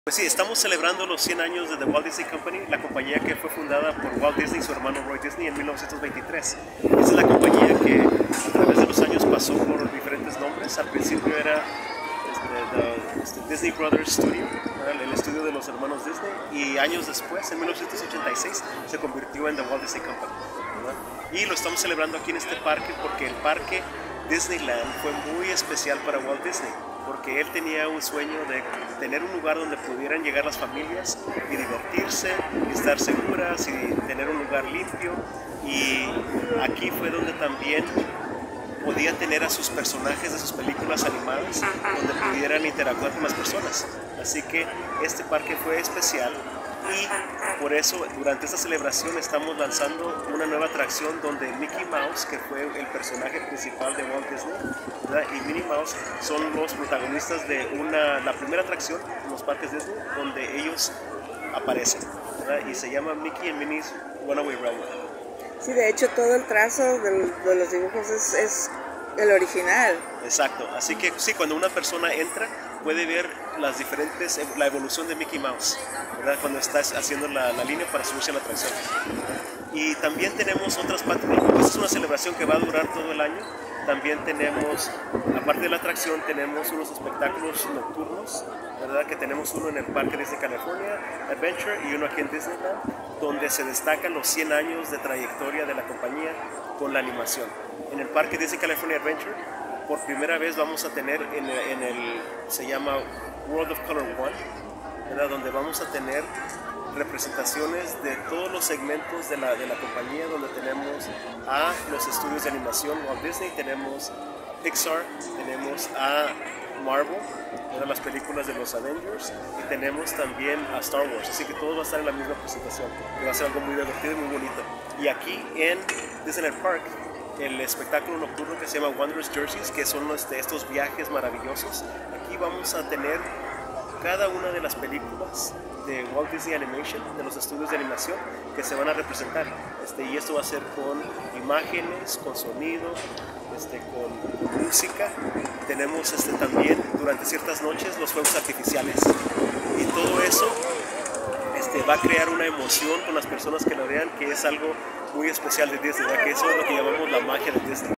100 años de historias de Walt Disney Animation Studios en el Disneyland Resort
El 16 de octubre de 1923 Walt y Roy Disney fundaron los Disney Brothers Studios, piedra angular de lo que hoy son conocidos como los Walt Disney Animation Studios.
Los hermanos Disney pronto lideraron la industria de los dibujos animados con la popularidad de Mickey Mouse, personaje que estelarizó “Steamboat Willie”, la primera película de caricaturas en integrar sonido en la etapa de post-producción.
Con más de 60 largometrajes en su haber, a partir, claro, de “Blanca Nieves y los 7 enanitos”, la compañía con sede en Burbank, California, ha sido una fuente inagotable de sueños hechos realidad.
Y es esto precisamente el motivo de una grandiosa celebración que el viernes 27 de enero comenzó a brillar en plateado en el Disneyland Resort honrando el primer siglo de los Walt Disney Animation Studios, tal y como lo explica en entrevista con ZETA Evan Palomeque, vocero de los parques de diversiones en “El lugar más feliz de la Tierra”. Gabriela Olivares.